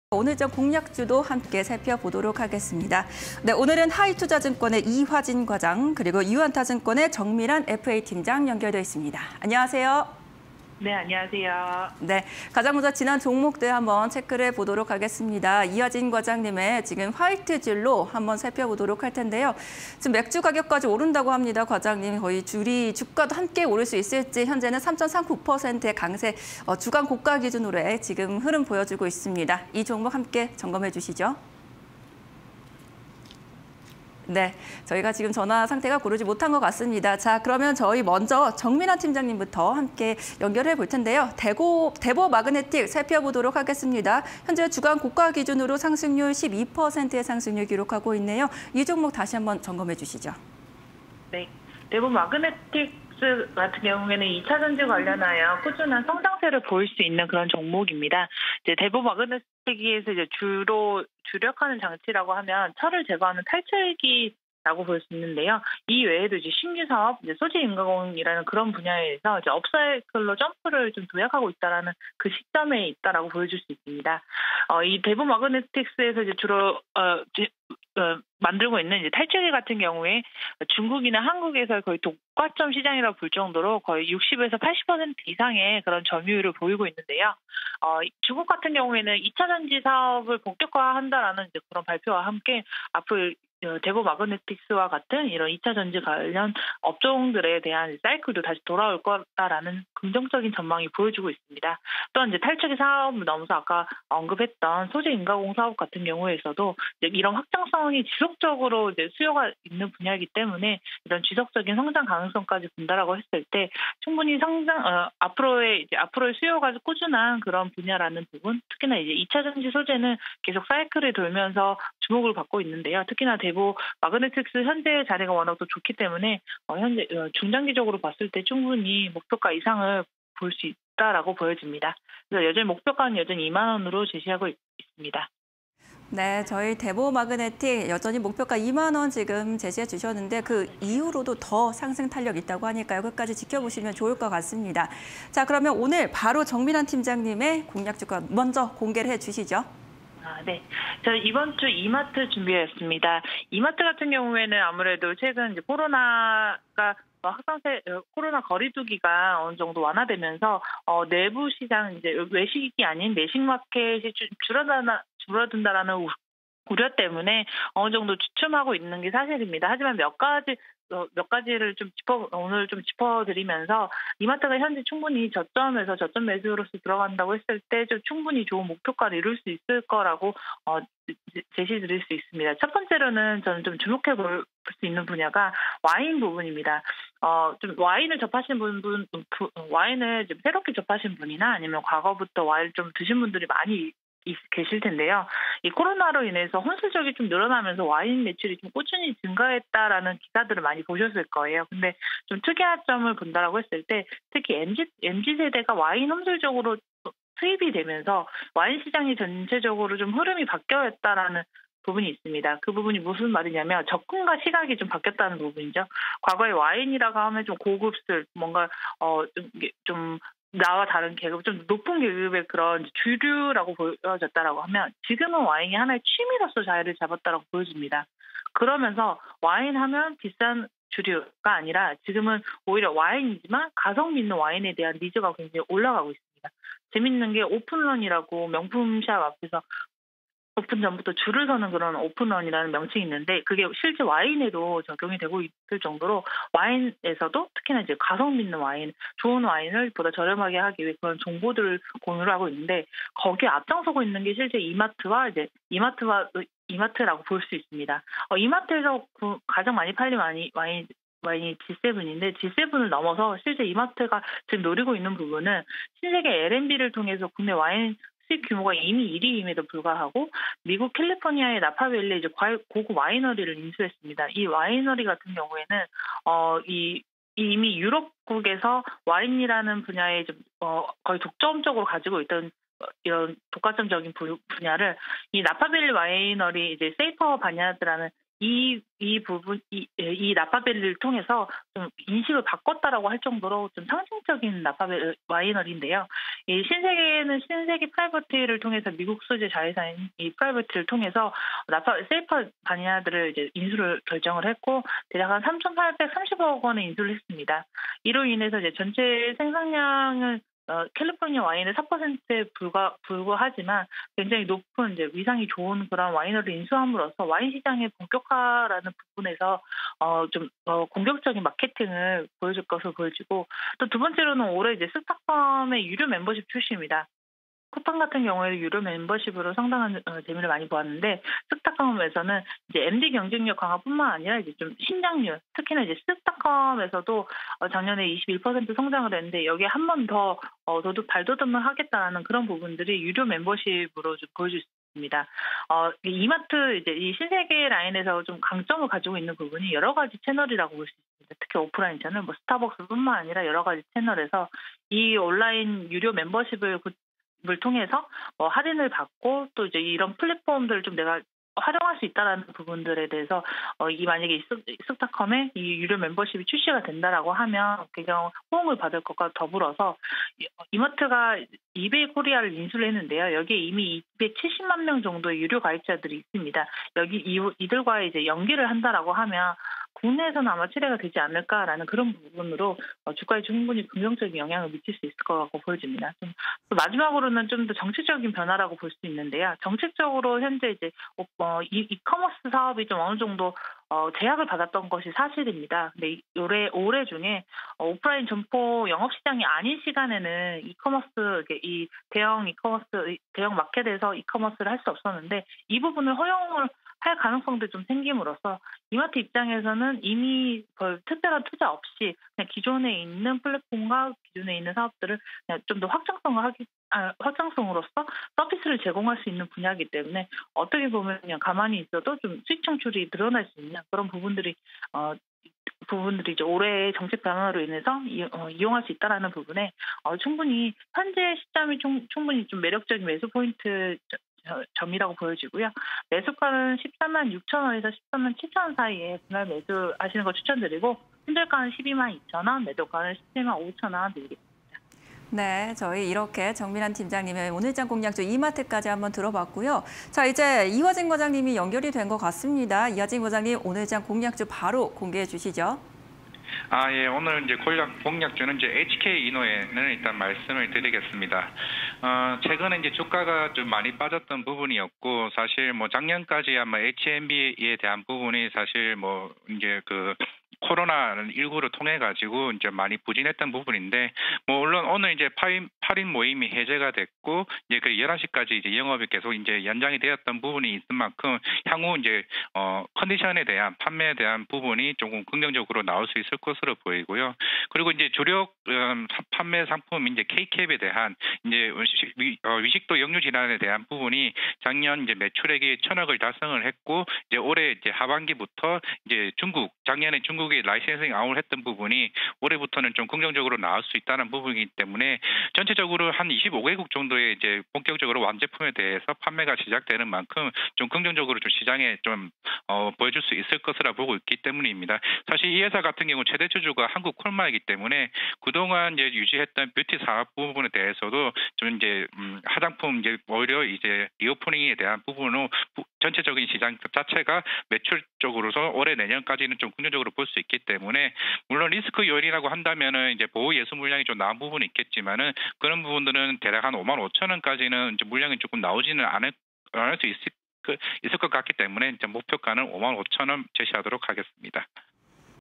HK이노엔(195940) VS 이마트(139480)_종목 VS 종목 (20220318)
HK이노엔(195940)
- 위식도역류질환 치료제 ‘케이캡’ 中 승인 임박
- 케이캡, 美 진출 시동… 성장 기대감↑
- 거리두기 조정 통한 HB&B 사업부 매출 증대
- 머크사와 백신 협약 통한 추가 계약 기대

이마트(139480)
- 이마트24, 주류특화매장 확대
- 와인 객단가 상향… 전문점 수준 와인 구비
- 가성비 있는 와인 매니아층 공략
- SSG닷컴 19.6% 성장… 온라인 시장 성장률 상회
- SSG닷컴 유료 멤버십 출시 예정
- 1~2월 이마트 할인점 전년대비 3.1% 성장